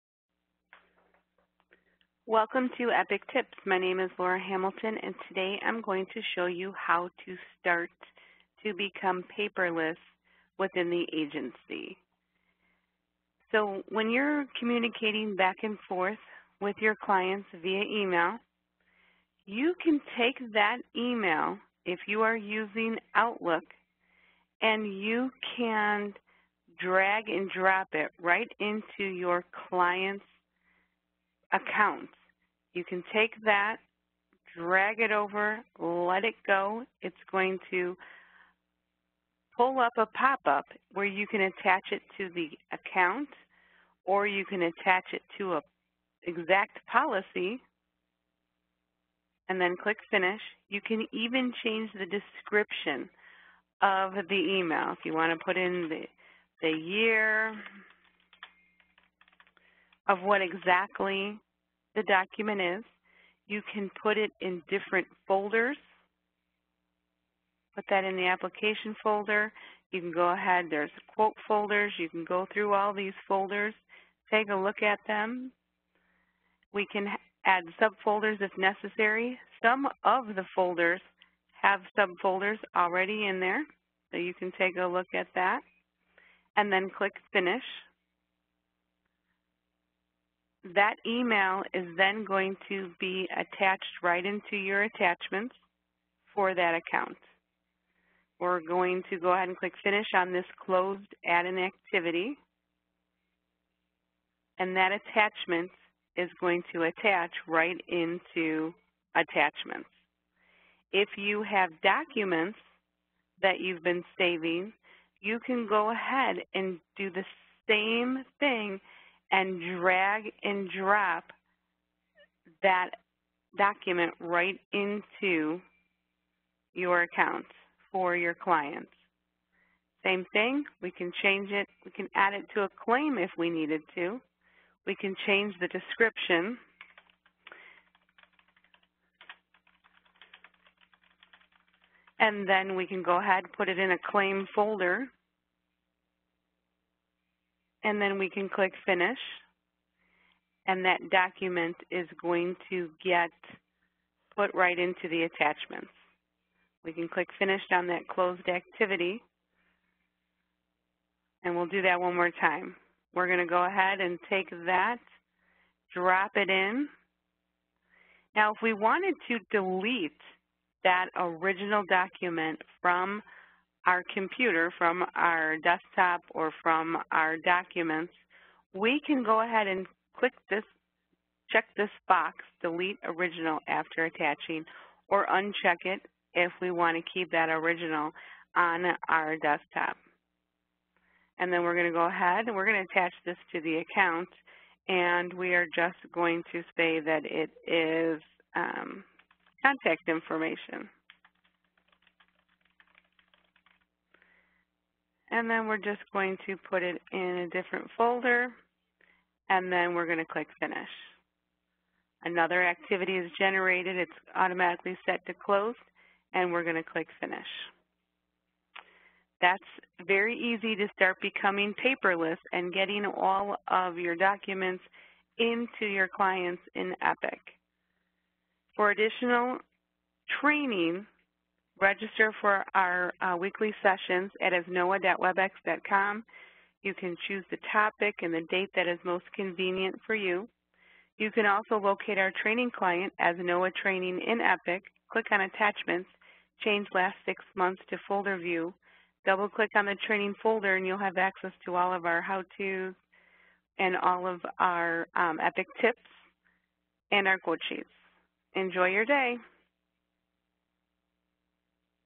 How to - Start being paperless in Epic
Learn how to navigate through the attachments area in Epic and ultimately become paperless!